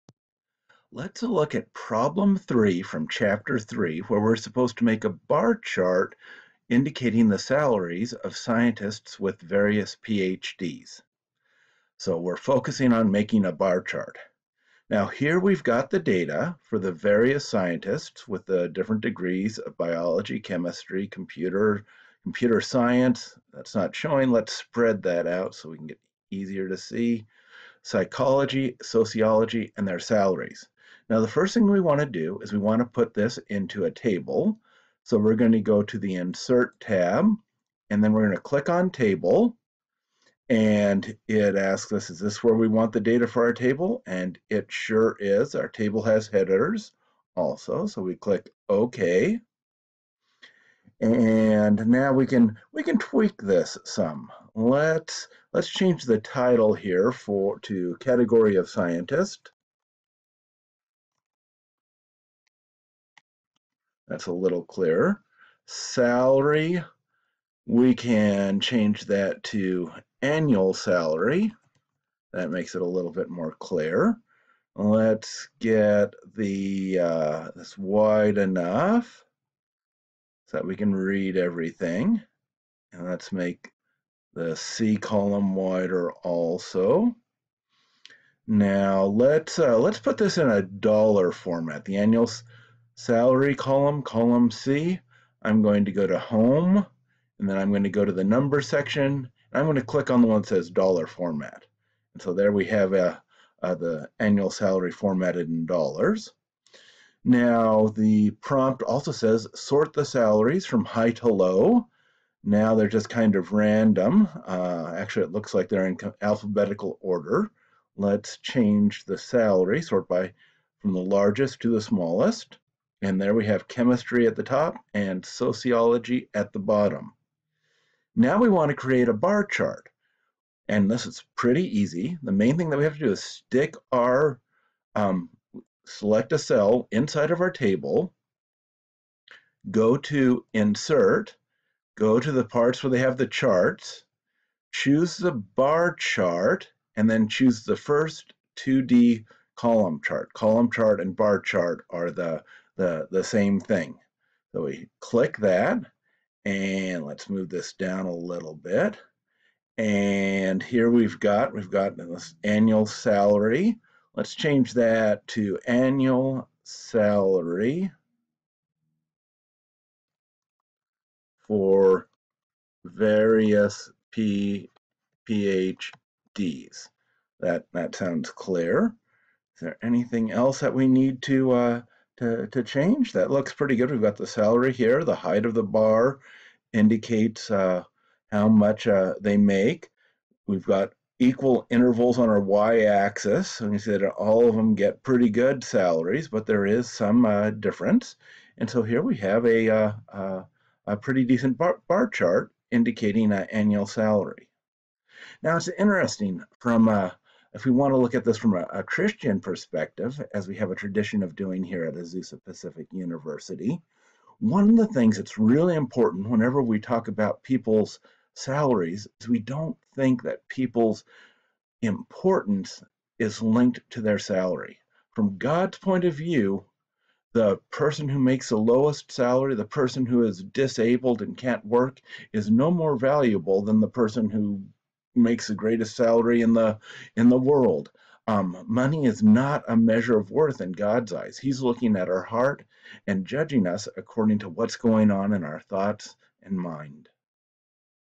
Chapter 3 Problem 3 from Pagano's Understanding Statistics using Excel, HW2
This video shows how to do Problem 3 from Chapter 3 of Understanding Statistics in the Behavioral Sciences by Robert Pagano using Excel. It is for Homework #2 of OPSY 590 at Azusa Pacific University.
The second half of the video contains a personal reflection linking a concept in the problem to a biblical concept. If this does not interest you, please skip this part.